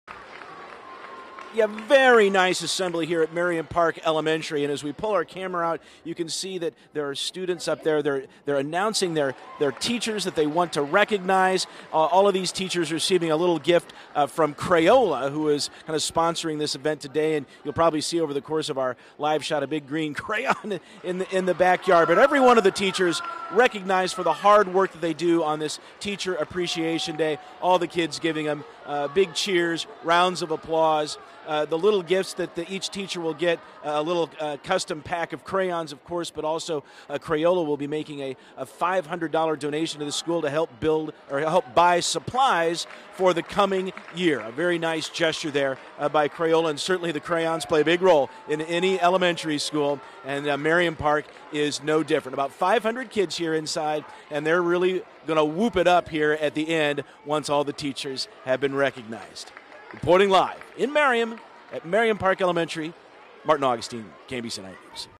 Crayola recognizes teachers at Merriam Park Elementary School
Teachers at Merriam Park Elementary School  were recognized with small gifts from Crayola on Teacher Appreciation Day.